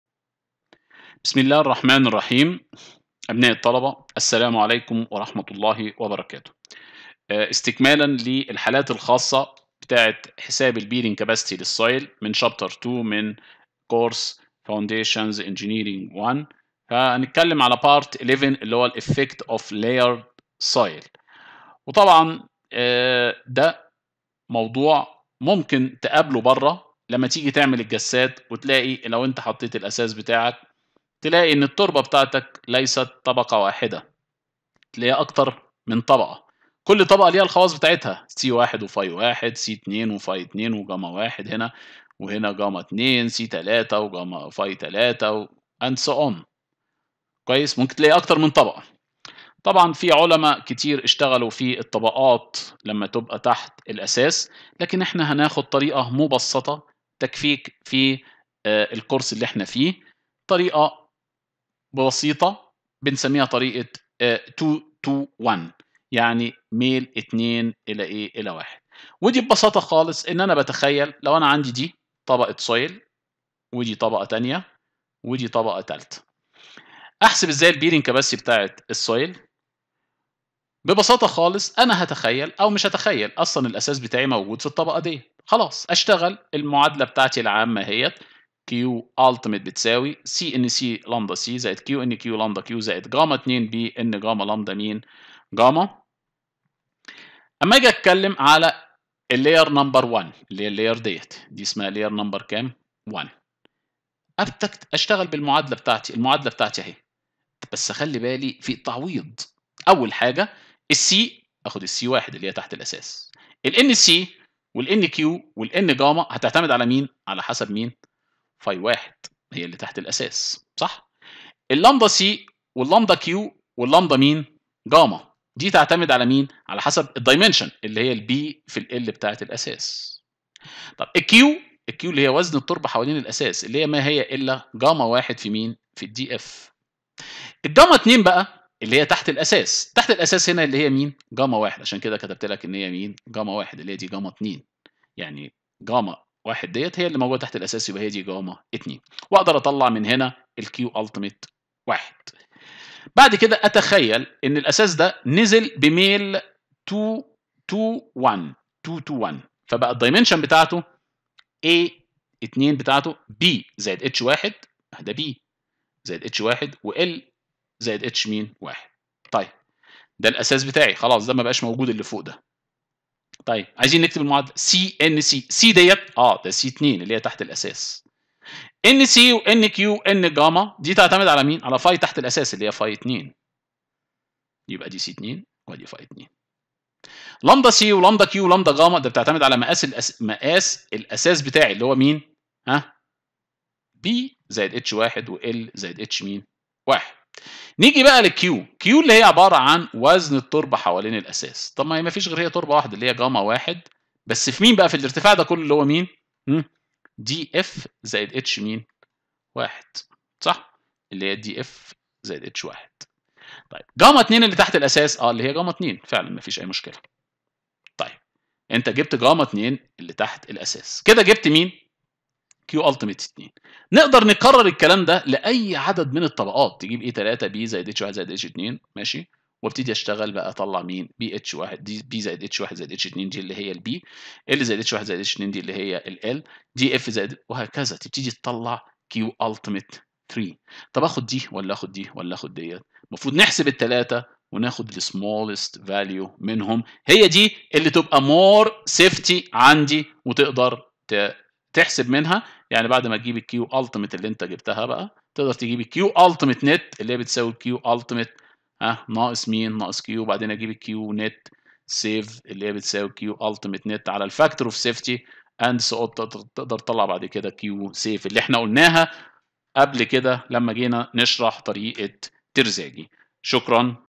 Foundations Engineering 1 | C2-L11 | Effect of Layered soil
Chapter 2 : Bearing capacity of soil
Lecture 11 : Effect of Layered soil
Explain Effect of Layered soil in calculation Bearing capacity of soil